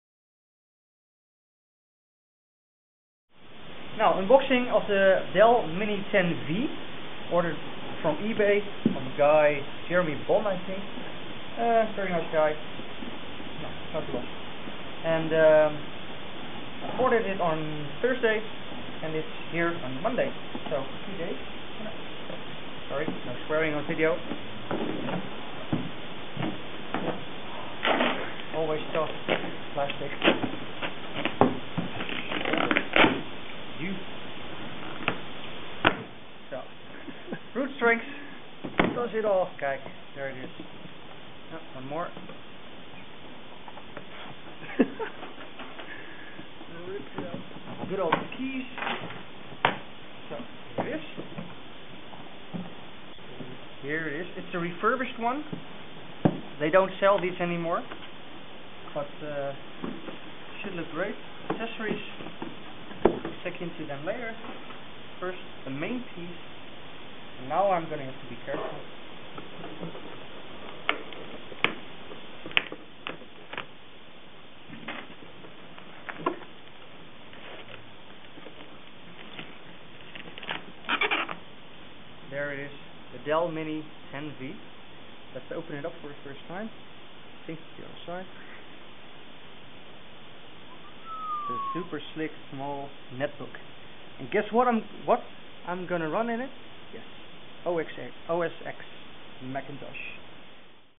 Dell Inspiron Mini 10v netbook unboxing
A few day's ago I bought a Dell Inspiron Mini 10v from eBay. I wanted to use it to install OS X Snow Leopard. I was excited like a child with a christmas present